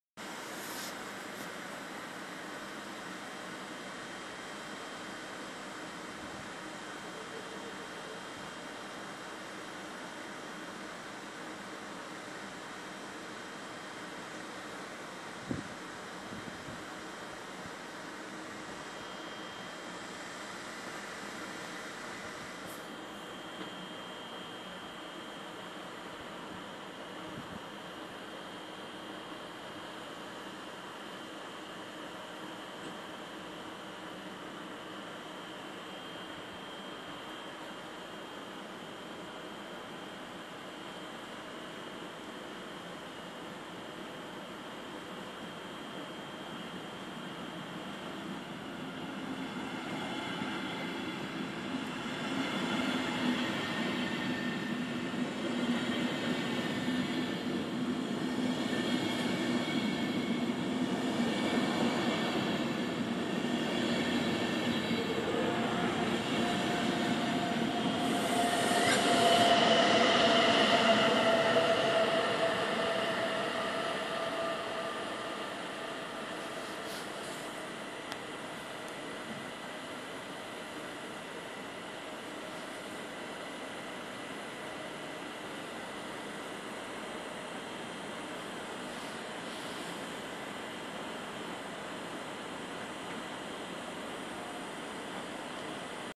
Raus in die Landeshauptstadt von Sachsen
Mir begrüßen Sie recht herzlich im ic nach Dresden hbf über Riesa Dresden Neustart und wünschen Ihnen eine angenehme fahrt usw.